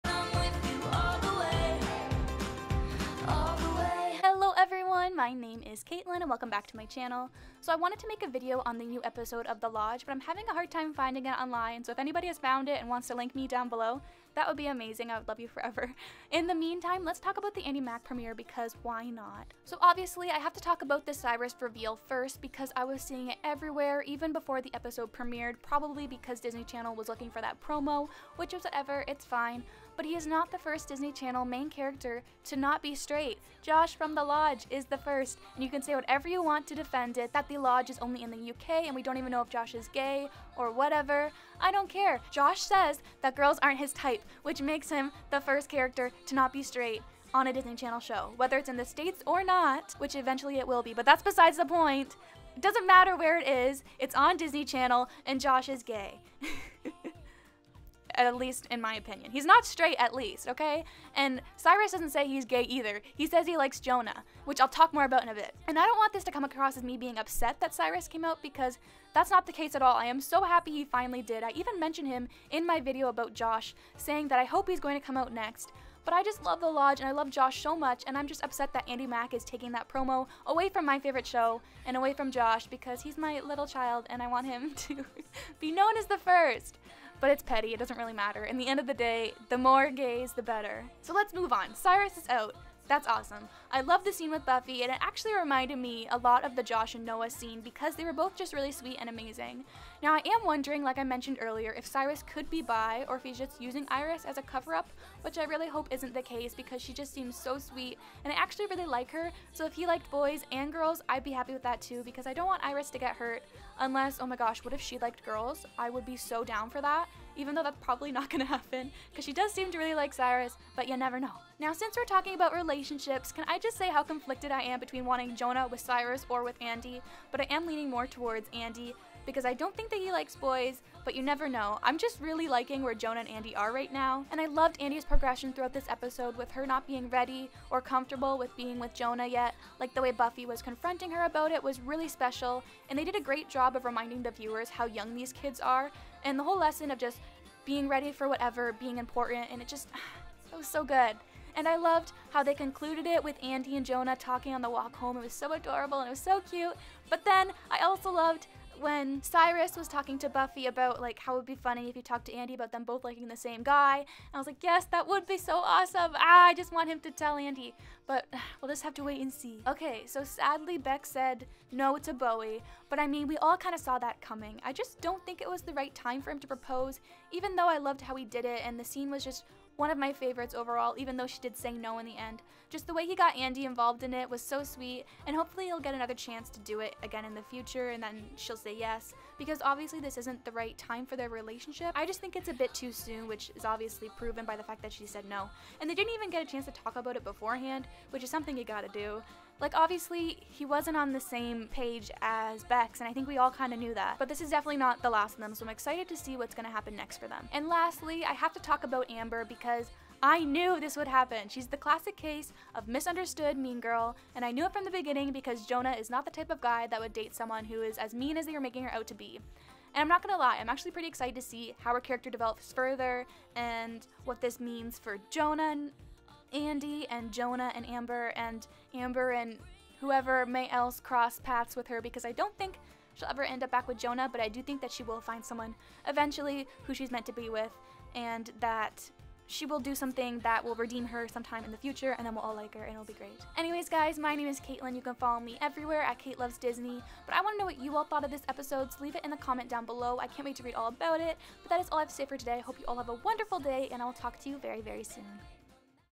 Cyrus Is NOT First To Come Out on Disney Channel!! Andi Mack "Hey, Who Want's Pizza?" Review!!
I do love him though.

let's be pen pals!! 🥰
Caitlin McKillop
Broadway PO PO
Toronto, ON, CA
M4P 0A8